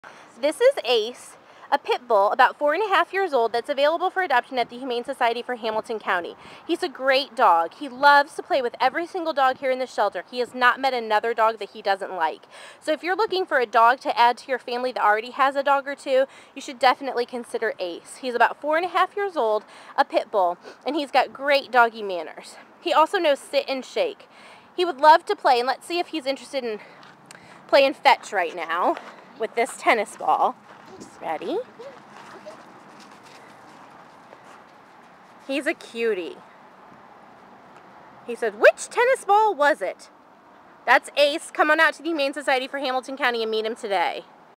Ace.AVI
INTRODUCING ACE --- the most dog-friendly dog at HSHC!!

He loves to play with all other dogs...and doesn't know a stranger!  He would make a great member of a family that already has dogs.  You'll fall in love when you meet this guy!